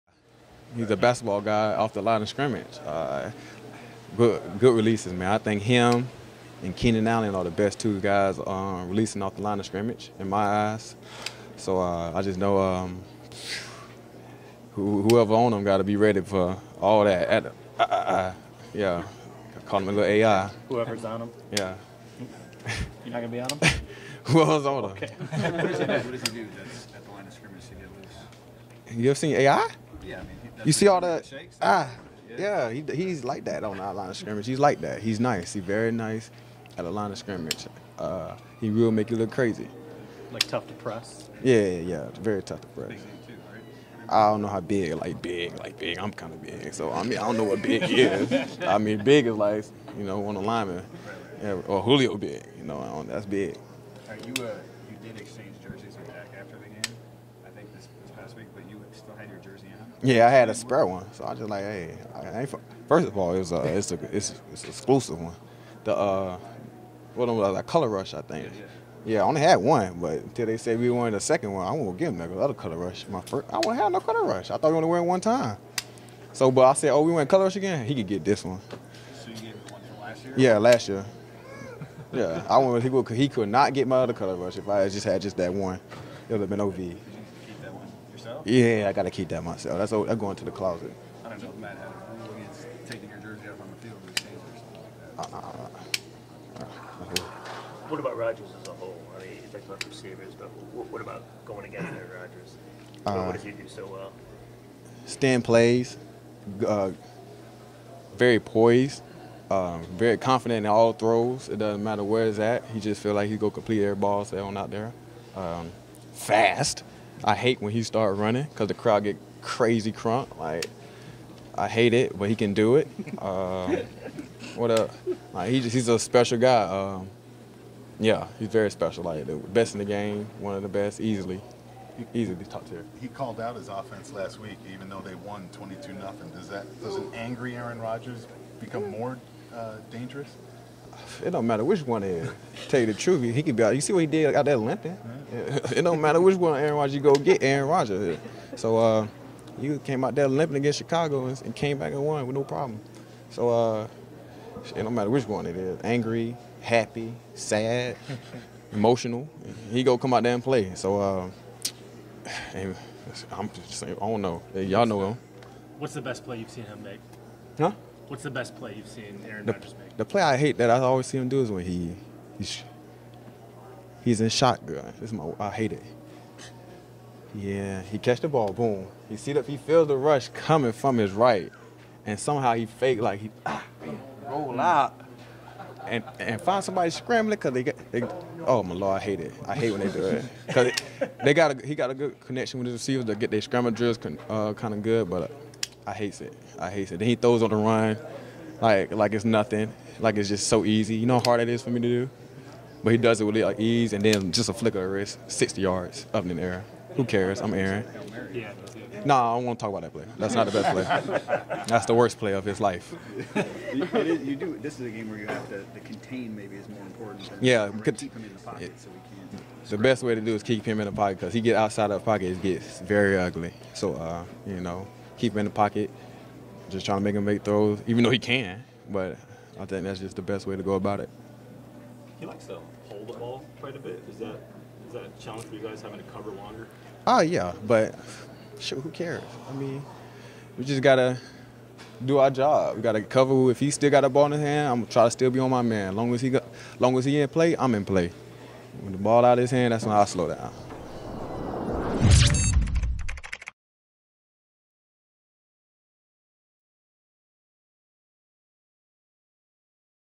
Darius Slay on Green Bay
CB Darius Slay discusses matching up against some of Green Bay's key players.